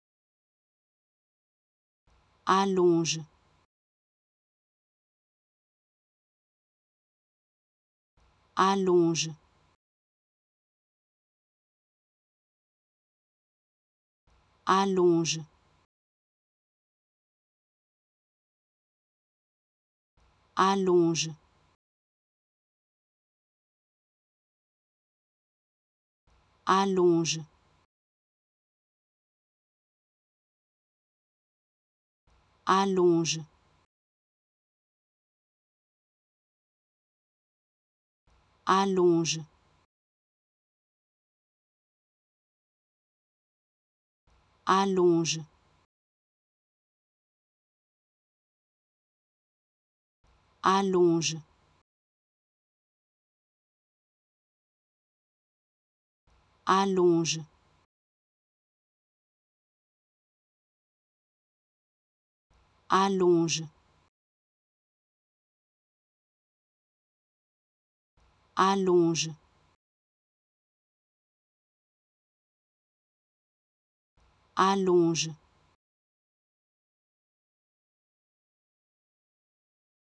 How to Pronounce ''Allonge'' (Lengthening) Correctly in French
Learn how to say and properly pronounce ''Allonge'' in French with this free pronunciation tutorial. Apprenez à prononcer des mots en français grâce à nos tutoriels.